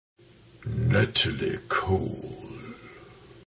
Natalie Cole Brief Tribute
Eric Butthoist talks about the great jazz diva Natalie Cole. The daughter of Nat King Cole
who is also briefly tributed by the talkitive Eric Butthoist.